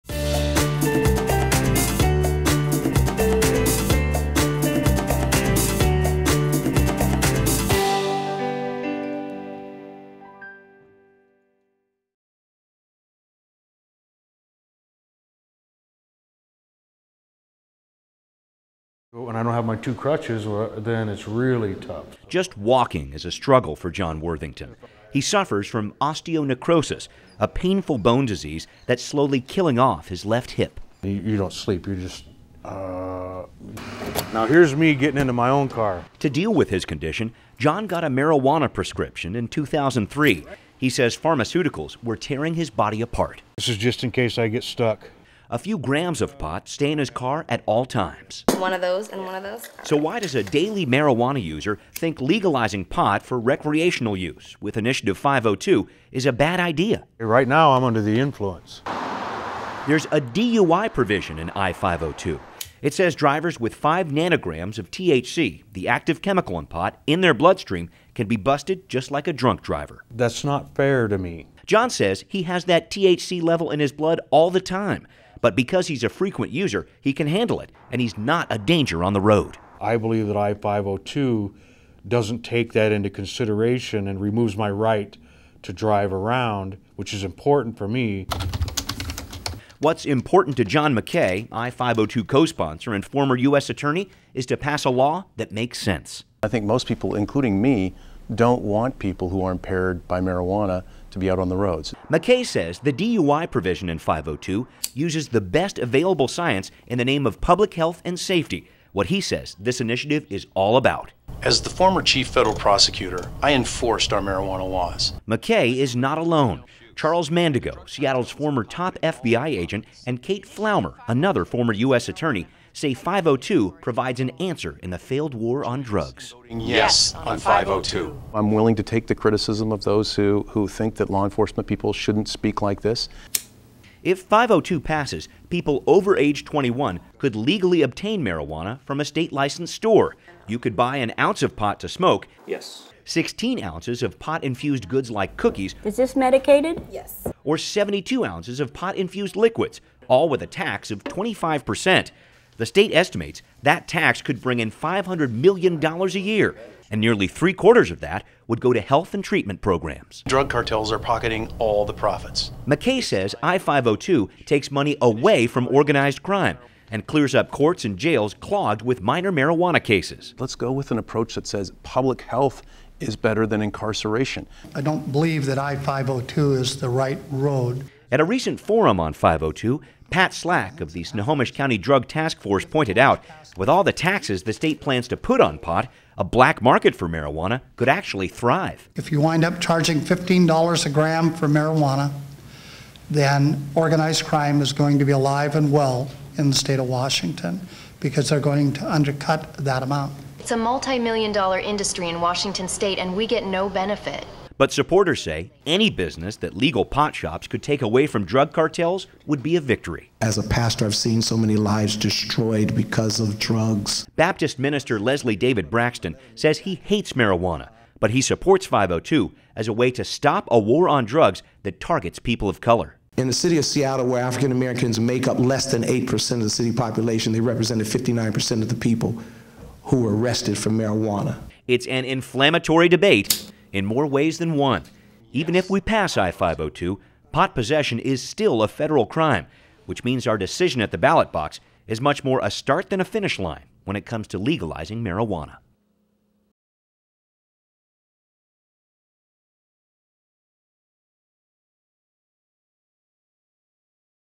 CIO Local Issues: Initiative 502 - Marijuana Legalization
On your ballot this fall, Initiative 502 is asking voters to legalize the recreational use of marijuana. And it`s not a pipe dream. I-502 is ahead in the polls and even has the support of some law enforcement leaders from around the state. Ironically enough, the most vocal opposition to I-502 comes from medical marijuana users, who say this measure makes them a target for police. So what`s your call on this cannabis controversy? You`ll hear from both sides of the story to help you make your choice.